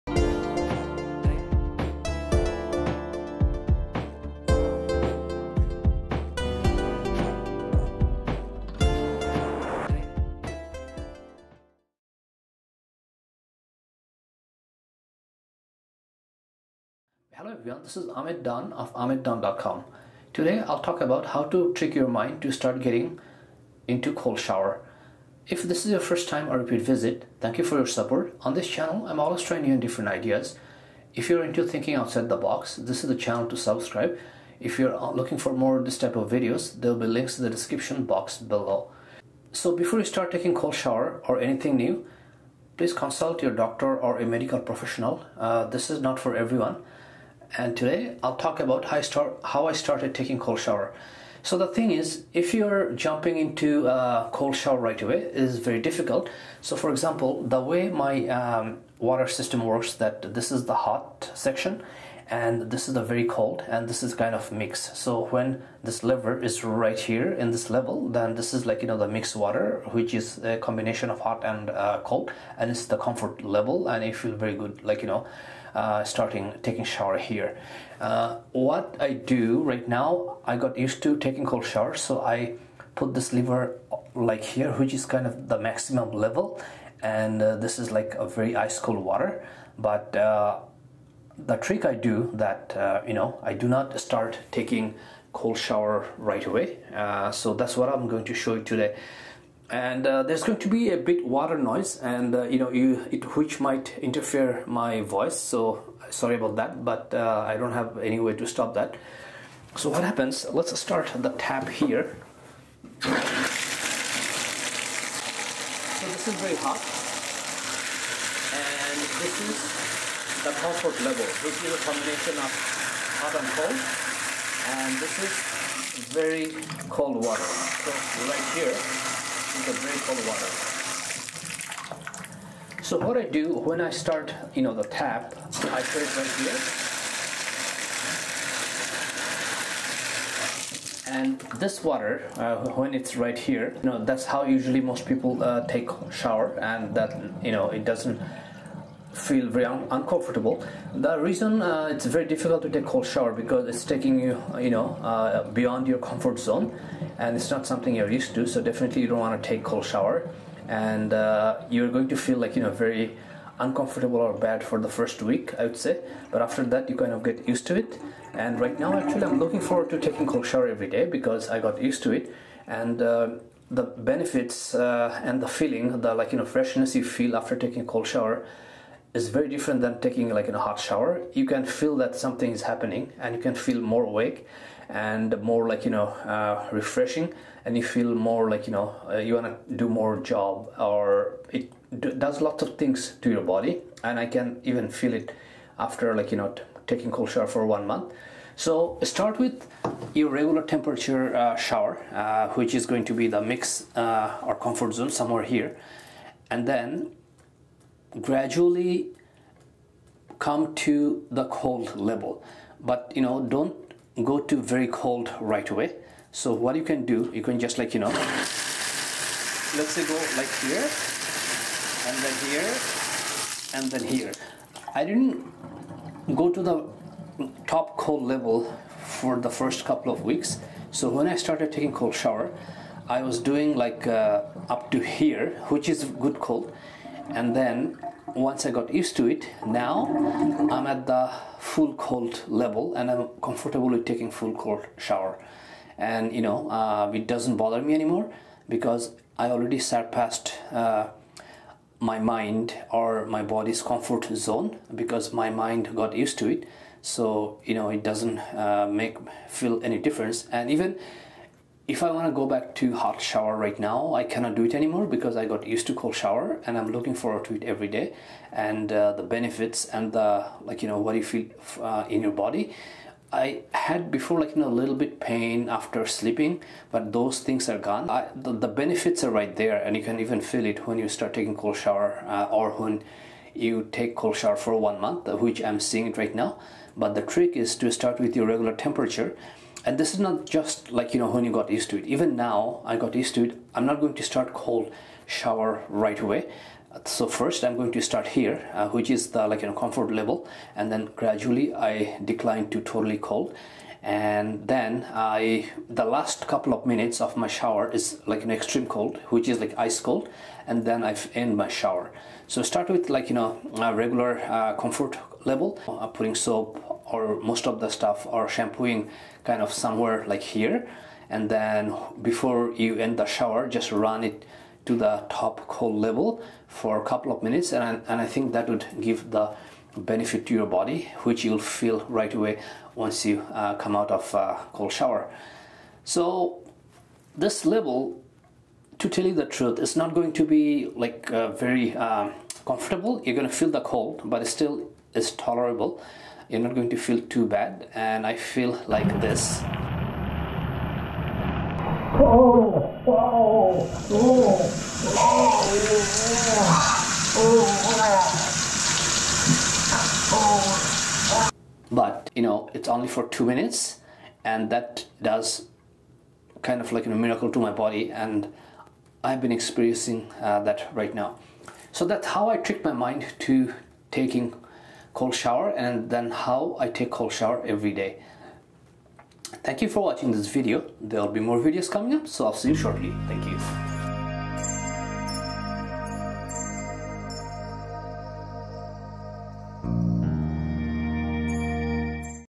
Mind Hack to Take Cold Showers | How to Take Cold Showers Daily
How I Trick My Brain to Take Cold Showers Every Day | Beginner's Guide to Start Cold Showers

Taking cold showers is no fun. You are leaving your comfort zone and embracing ice-cold waters, which your body doesn't like because your brain is telling so.

I felt terrible when I started taking cold showers. But the benefits surpassed all my tears and pain and now I can't or won't go back to a regular comfortable warm shower.

In this video, I will show you how you can easily take cold showers every day by taking some simple steps.

NB - Cold showers are not for everyone. Please consult your doctor or a medical professional before start taking cold showers.

Chapters:
0:00 Intro
0:11 Before Taking Cold Showers
0:50 How to take Cold Showers
3:20 2How to Trick Your Brain to Take Cold Showers
5:45 Don't Be Afraid to Scream